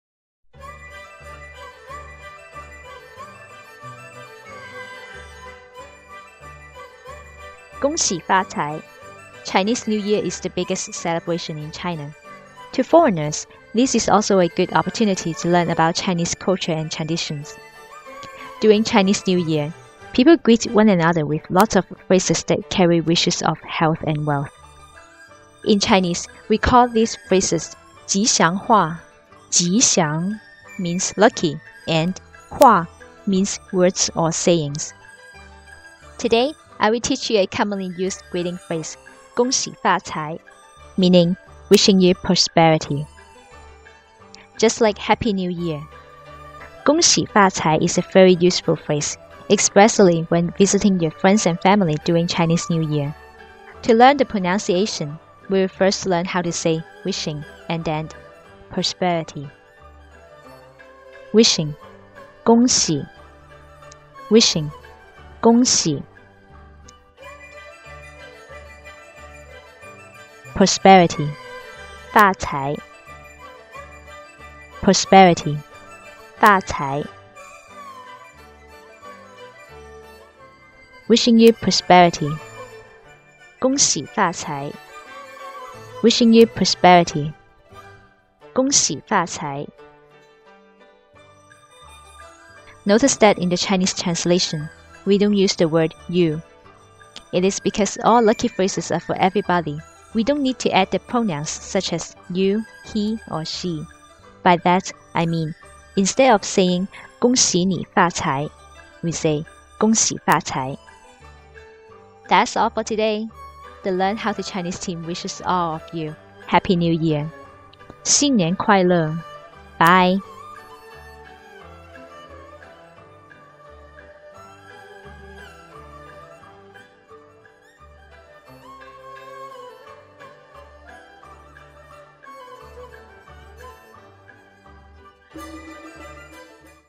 Learn How To Say The Greeting Phrase "gōng xǐ fā cái" in Chinese
In this lesson, you will learn a commonly used greeting phrase - 恭喜发财 [gōng xǐ fā cái], meaning "wishing you prosperity". During Chinese New Year, people greet one another with lots of phrases that carry wishes of health and wealth...